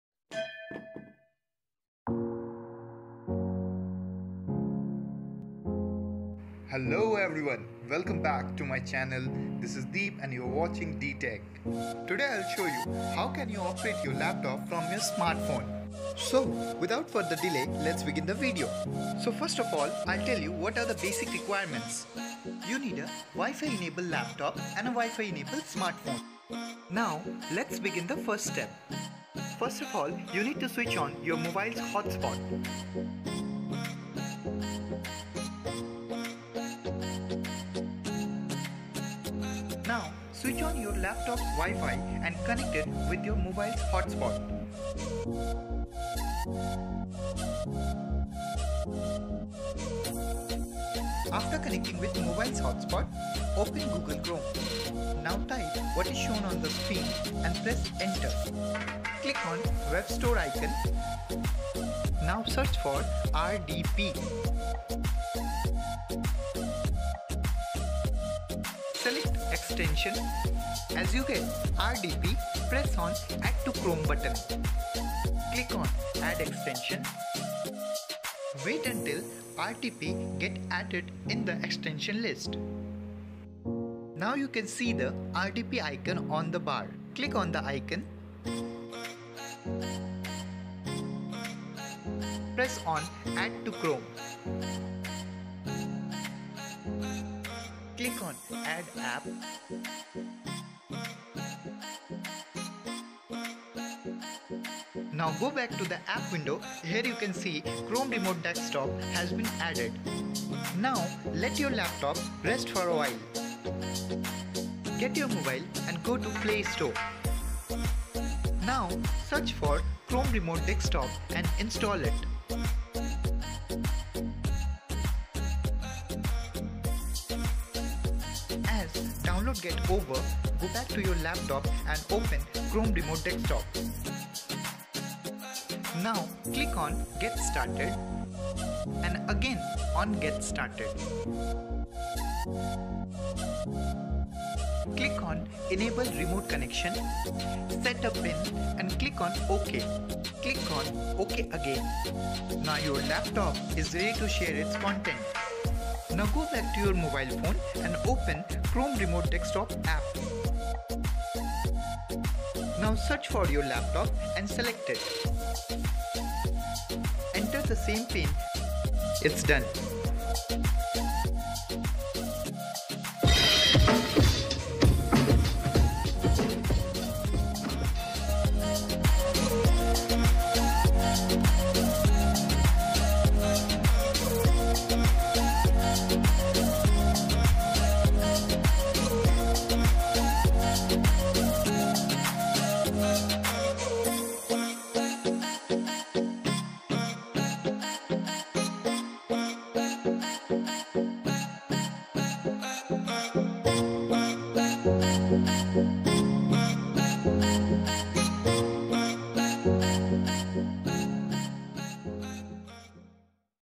How to Control Laptop from Phone || Cast Laptop to SmartPhone || Simple Trick
Cast or mirror your Laptop or Desktop to your Phone using Google Chrome Remote Desktop.
Control your Desktop or Laptop easily from your Smartphone with the help of Chrome Remote Desktop Application.

If you get any confusion then put your doubts on the comment section below!!!

Keep Supporting!!!